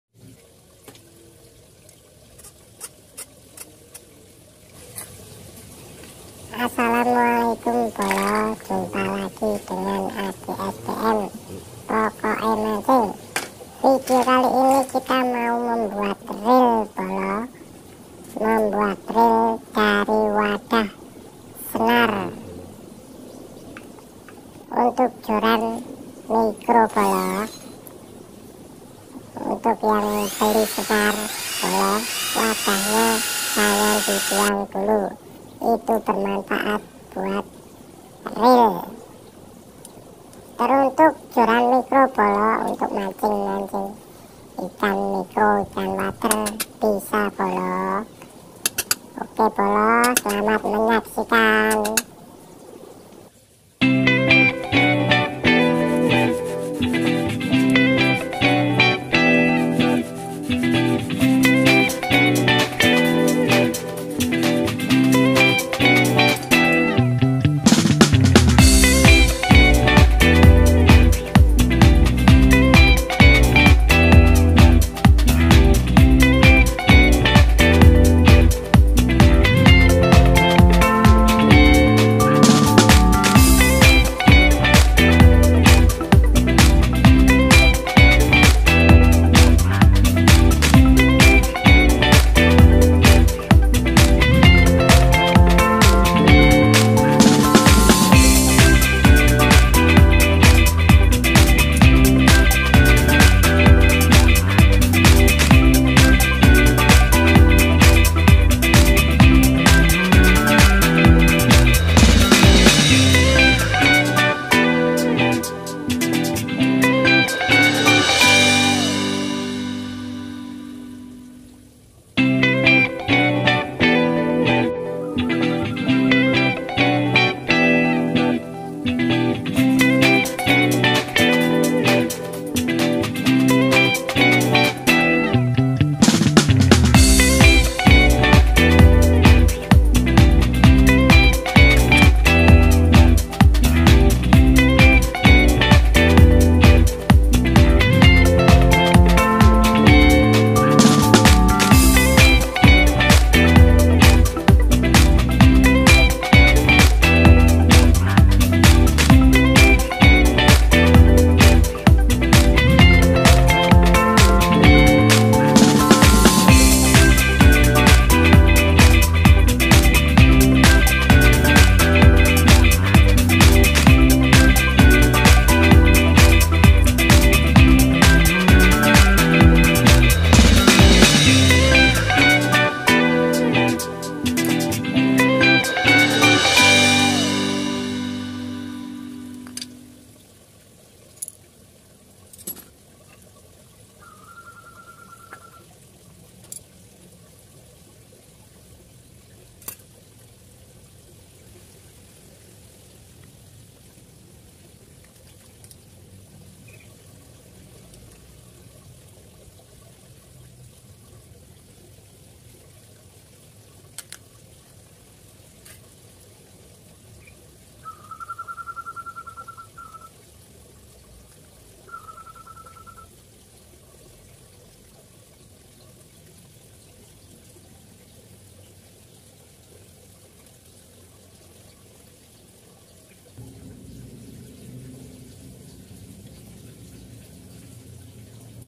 buat REEL PANCING DARI BEKAS WADAH SENAR
Membuat reel pancing sendiri yg terbuat dari bahan bekas wadah senar .membuat reel pancing untuk joran mikro, bekas wadah senar buat reel pancing